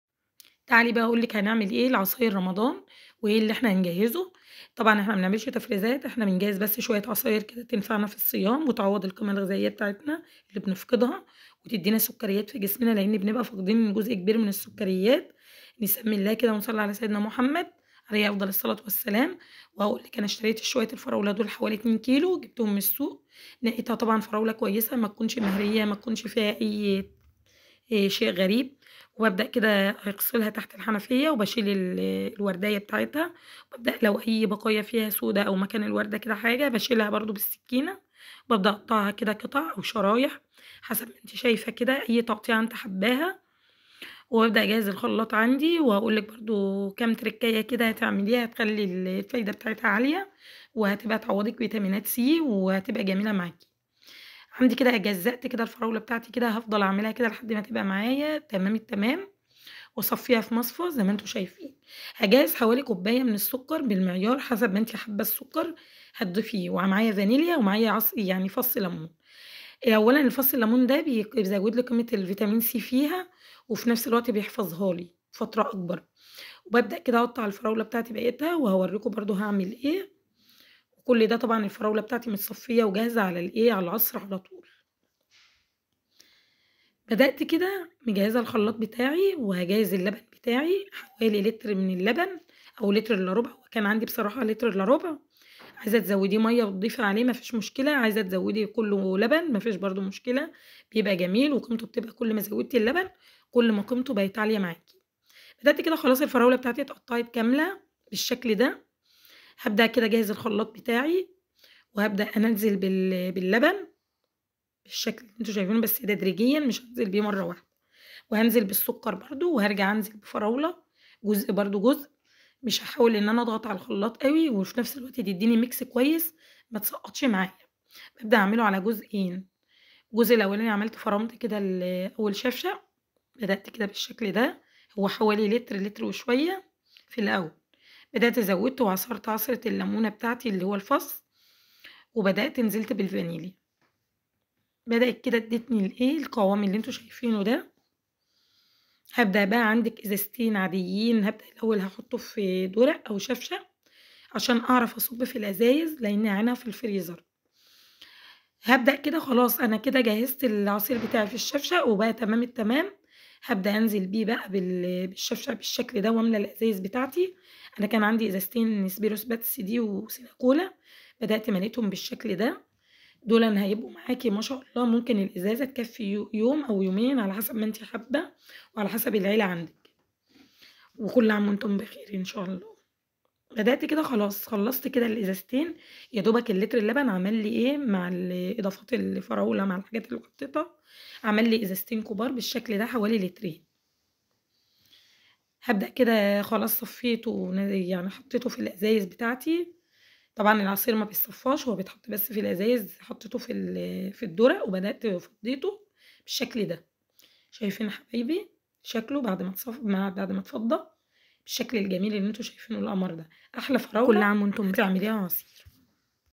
إستعدى_لشهر_رمضان بقلب جامد وأحلى عصيرهيعوضك الصيام بقيمه غذائيه عاليه#بميكس_اللبن_والفراوله
أحلى❗️ عصير ❗️لتقوية❗️ العظم وفيتامين سى